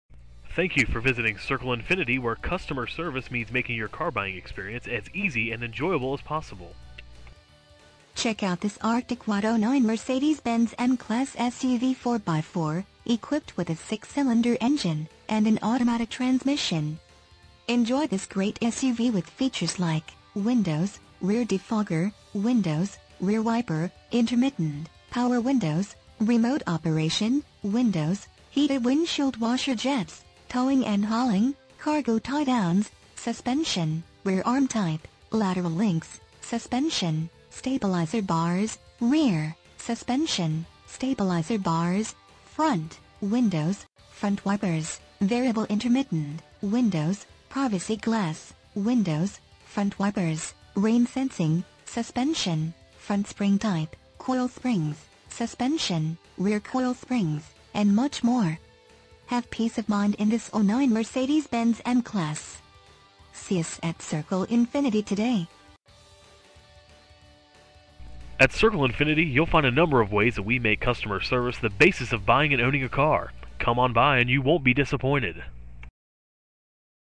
2009 Mercedes-Benz M-Class Circle Infiniti West Long Branch, NJ 07764
Mercedes-Benz M-Class You will love this Arctic White Mercedes-Benz M-Class SUV 4X4, equipped with a 6 Cylinder engine and an automatic transmission.
MILES: 32,556
VIN:4JGBB86E59A460469

Circle Infiniti
301 Highway 36
West Long Branch, New Jersey 07764